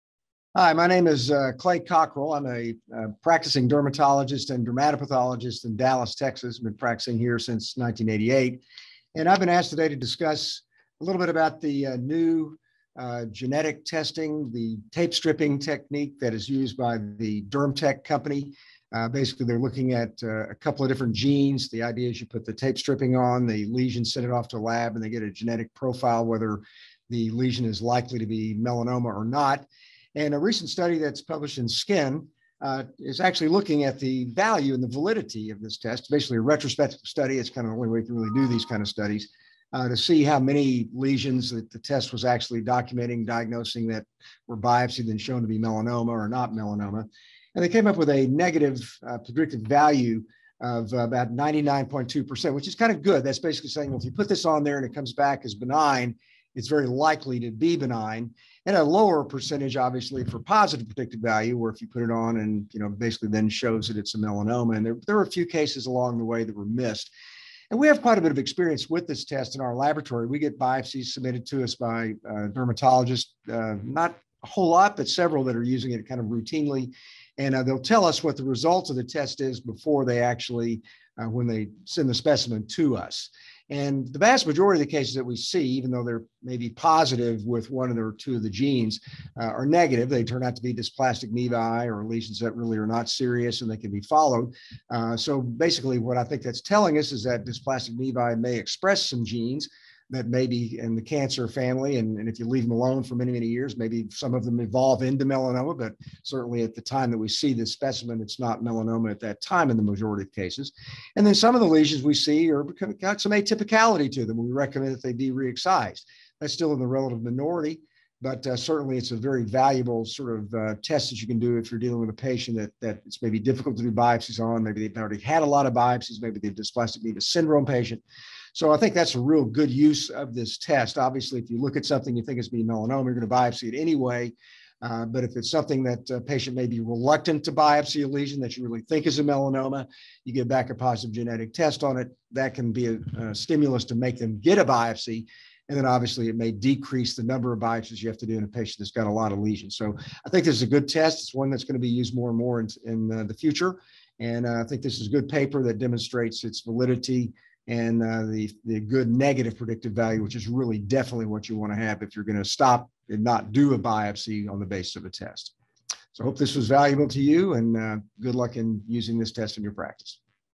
Clay J. Cockerell, MD - Non-Invasive Detection of Genomic Atypia Increases Real-World NPV and PPV...
In this video, Clay J. Cockerell, MD, Founder of Cockerell Dermtopathology in Dallas, TX, discusses a recent article posted in SKIN: Non-Invasive Detection of Genomic Atypia Increases Real-World NPV and PPV of the Melanoma Diagnostic Pathway and Reduces Biopsy Burden.

Authors: M Skelsey, B Brouha, J Rock, M Howell, B Jansen, L Clarke, G Peck.